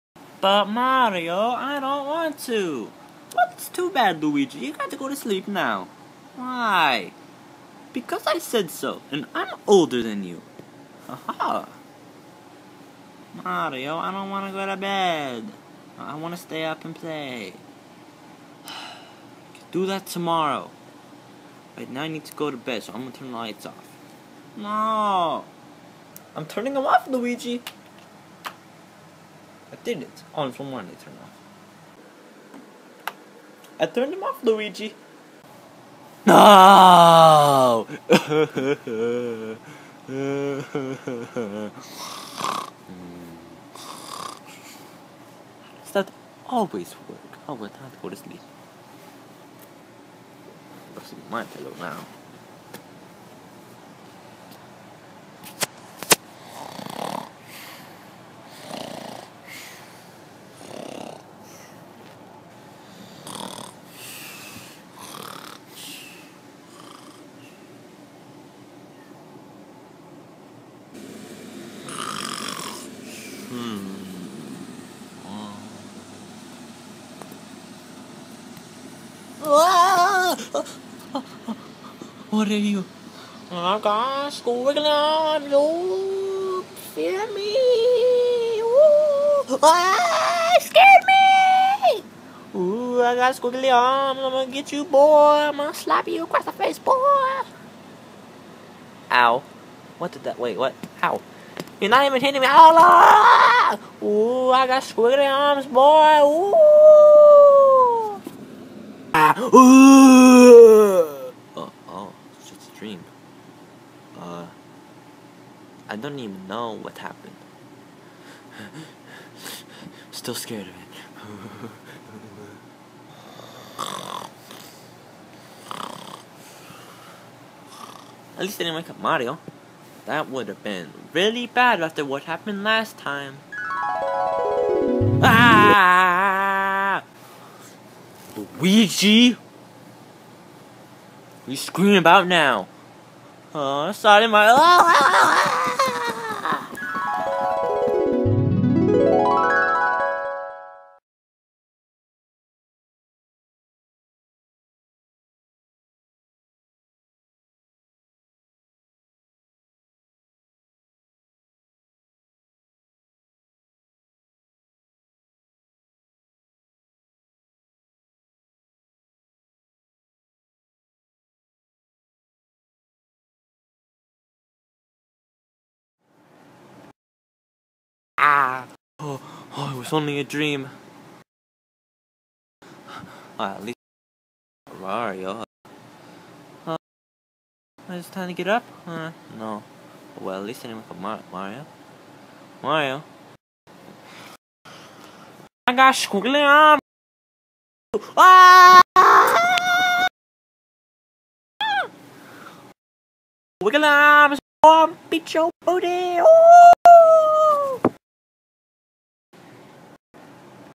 The B Mario Shorts Episode 23; Time for Bed 2
Mario bosses Luigi around and says he needs to go to bed.